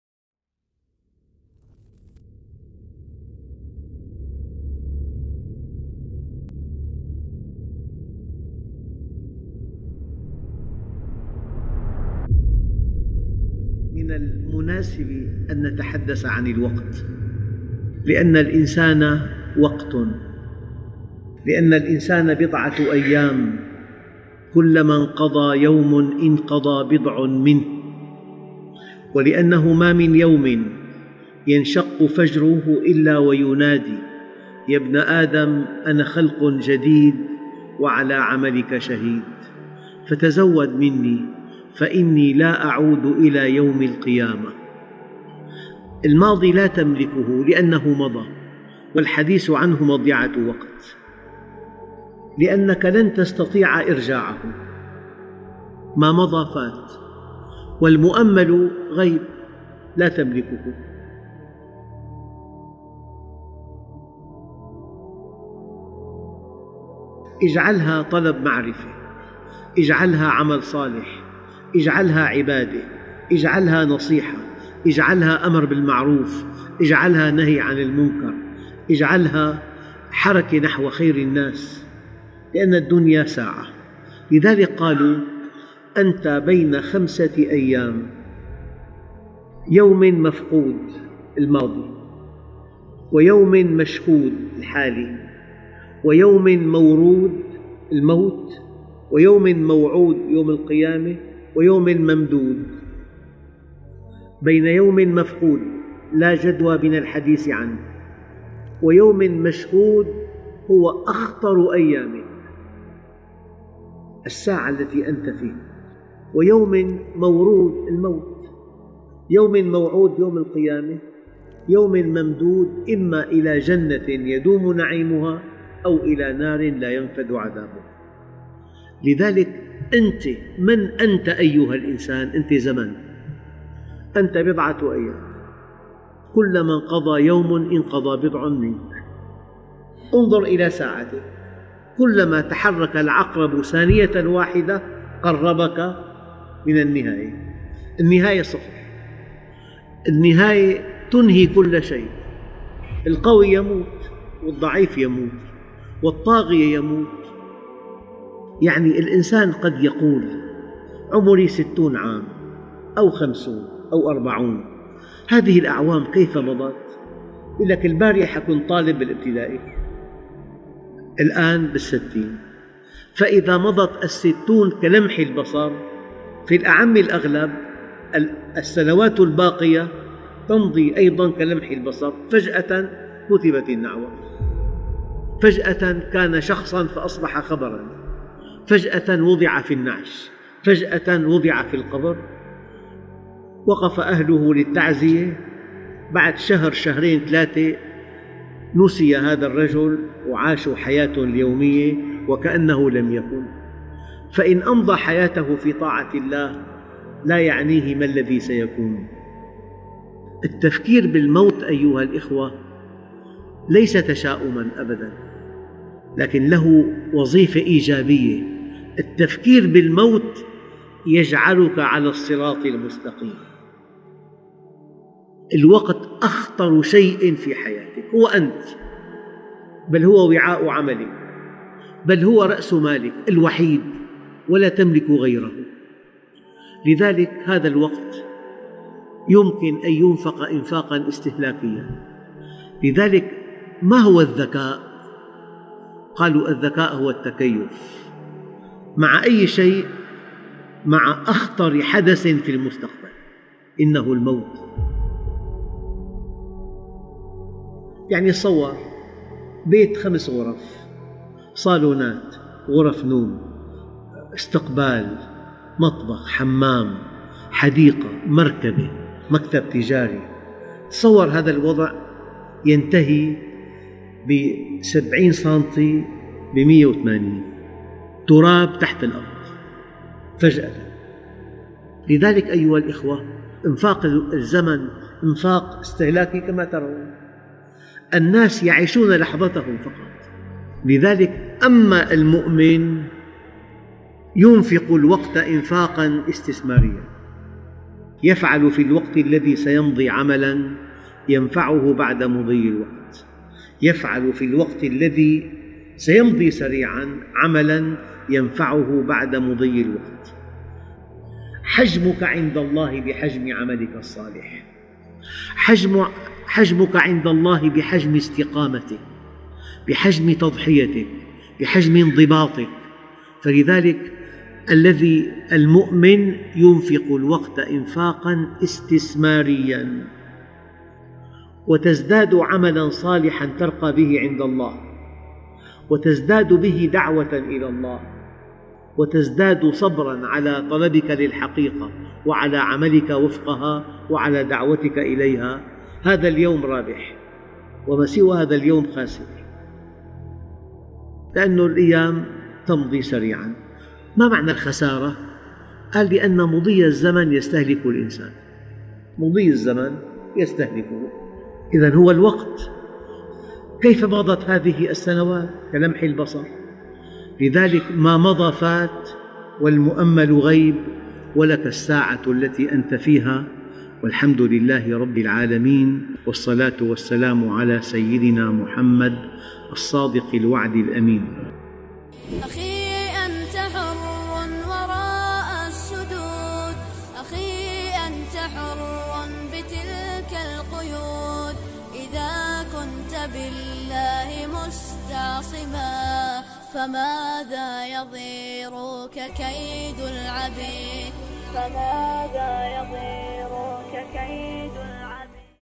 Time - A precious commodity in your life.
A heart touching Islamic reminder about time and it's impact on our lives.

Allah (SW) says in the Quran:
By  Time, Verily Man is in loss, Except who have Faith, and do righteous deeds, and (join together) in the mutual teaching of Truth, and of Patience and Constancy. (Quran 103:1-3)

A lecture by Mohammed Rateb al-Nabulsi.
Spread peace and love 🤍.